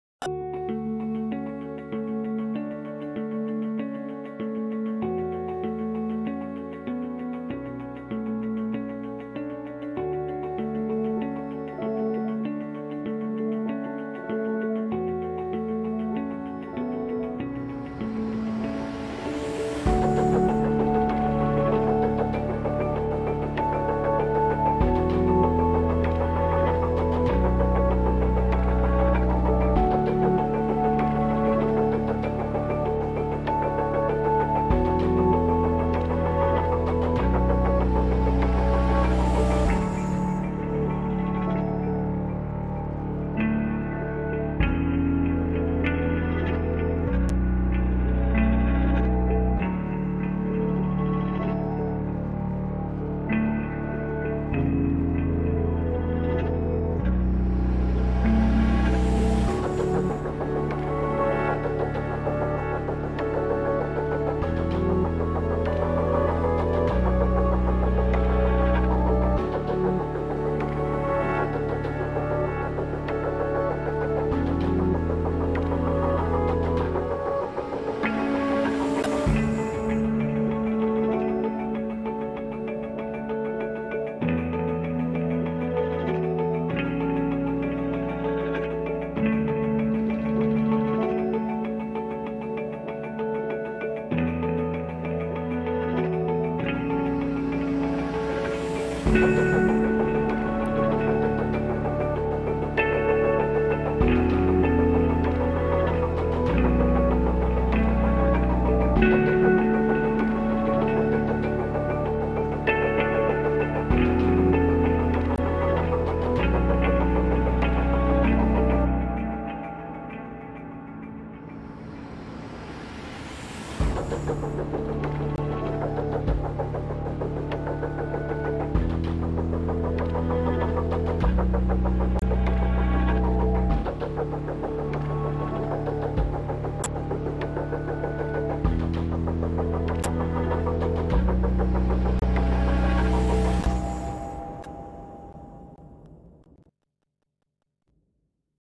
Светящиеся паруса и фейерверк в Нижнем Новгороде
15 августа 2020 года Нижний Новгород отметил свое 799-летие. В День города нижегородцы наблюдали фиесту воздушных шаров, шоу светящихся парусов в акватории Волги и Оки и праздничный фейерверк. В этом году салют был запущен прямо с воды, увидеть его можно было с городских набережных и откосов.
Видео: Владислав Базаев.
Монтаж: Андрей Журавлев.